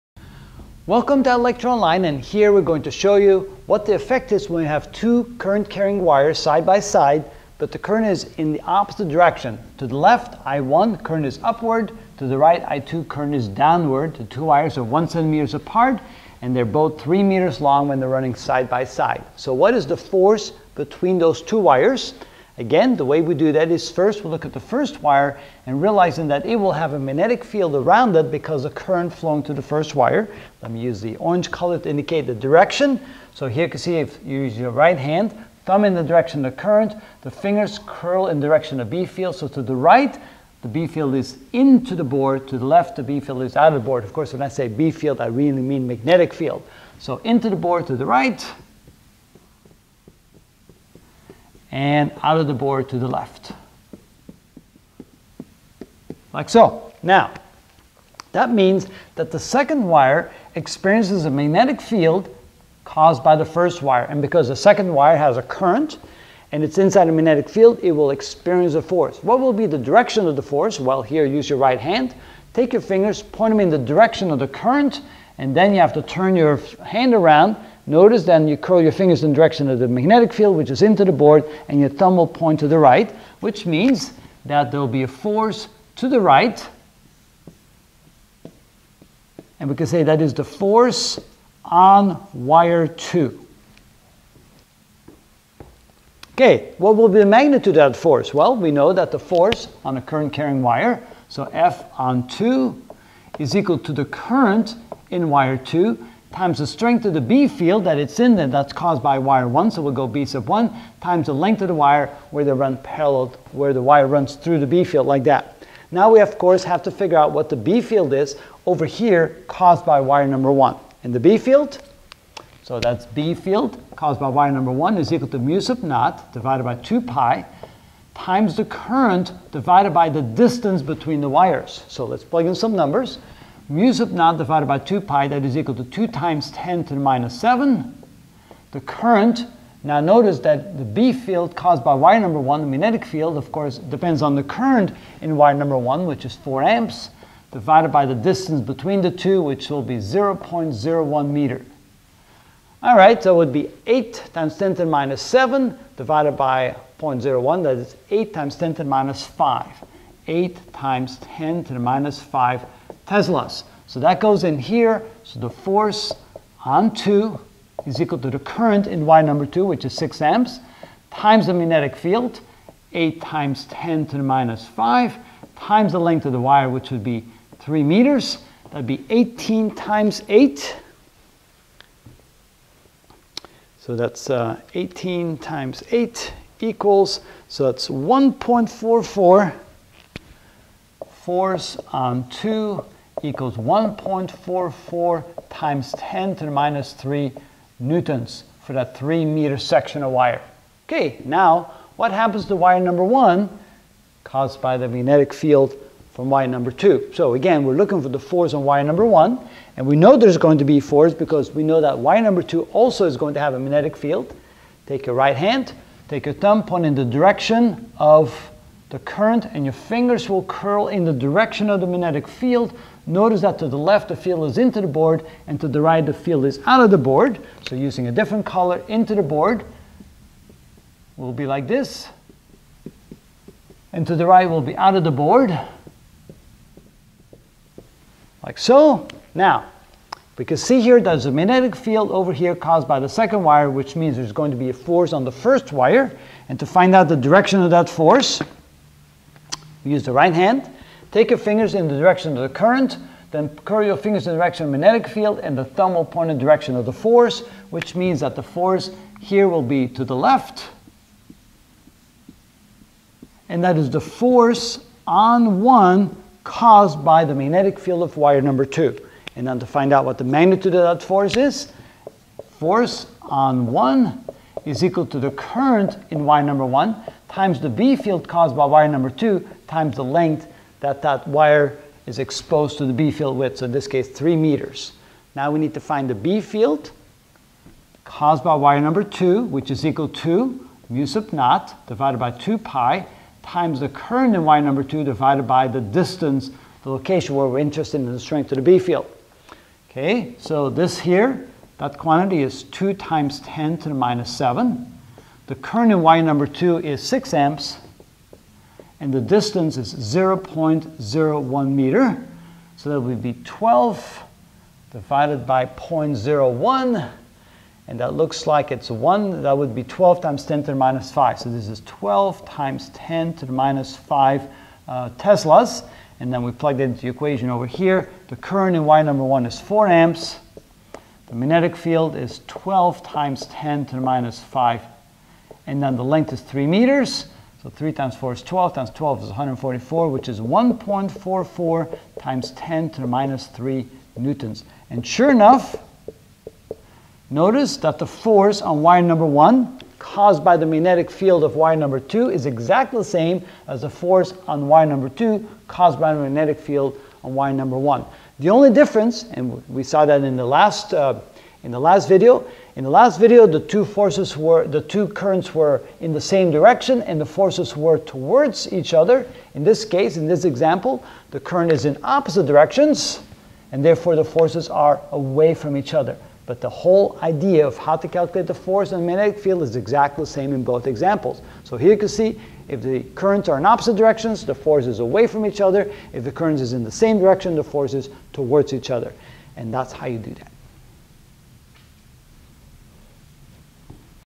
Physics 44 Magnetic Field Generated (20 of 28) F=? Parallel Conductors
In this video I will find the forces between 2 parallel conductors (current: opposite direction).